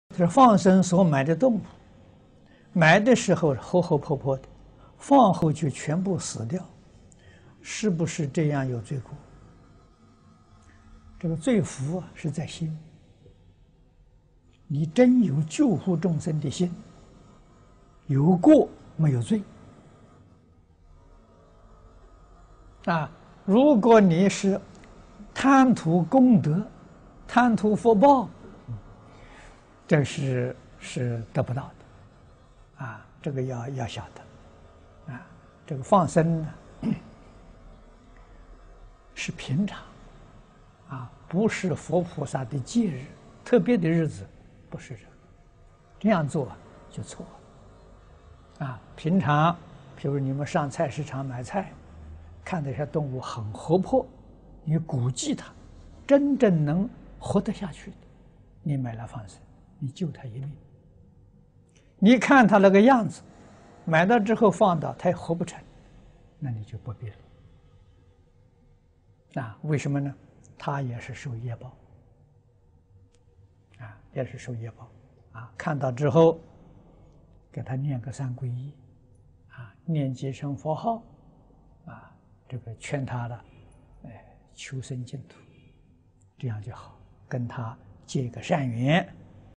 淨空老法師｜學佛答問｜動物放生後卻死了，是否有罪過？
🌿問：放生所買的動物，買的時候是活活潑潑的，放後卻全部死掉，是不是這樣有罪過？
🌿答：這個罪福是在心，你真有救護眾生的心，有過沒有罪；如果你是貪圖功德、貪圖福報，這是得不到，這要曉得。
放生是平常，不是佛菩薩的節日、特別的日子，不是的，這樣做就錯了。
平常，譬如你們上菜市場上買菜，看這些動物很活潑，你估計牠真正能活得下去的，你買來放生，你救牠一命。你看牠那個樣子，買來之後放了牠也活不成，那你就不必了。
為什麼？牠也是受業報，看到之後，給牠念個三皈依，念幾聲佛號，勸牠求生淨土，這樣就好，跟牠結個善緣。
2006/11/24